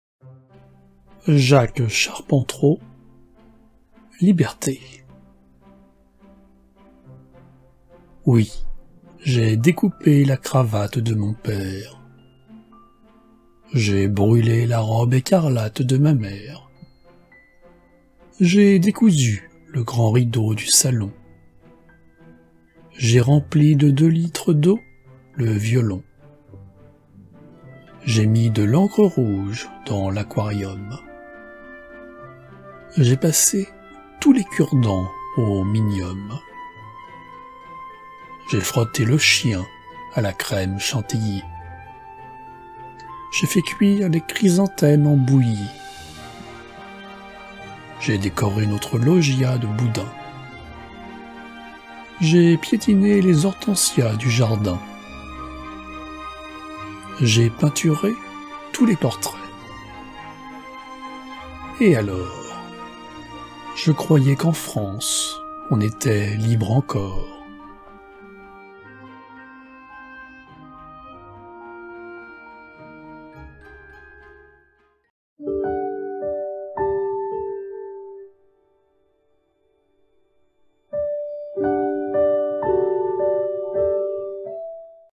Liberté ! - Poésie - Jacques Charpentreau
Liberté !
Poèmes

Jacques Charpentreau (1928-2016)

Illustration de fond : Fritz Zuber-Bühler

Texte :
Oui, j’ai découpé la cravate
De mon père,

J’ai brûlé la robe écarlate
De ma mère.

J’ai décousu le grand rideau
Du salon,

J’ai rempli de deux litres d’eau
Le violon.

J’ai mis de l’encre rouge dans
L’aquarium,

J’ai passé tous les cure-dents
Au minium.

J’ai frotté le chien à la crème
Chantilly,

J’ai fait cuire les chrysanthèmes
En bouilli.

J’ai décoré notre loggia
De boudins,

J’ai piétiné les hortensias
Du jardin.

J’ai peinturé tous les portraits.
Et alors ?

Je croyais qu’en France on était
Libre encor !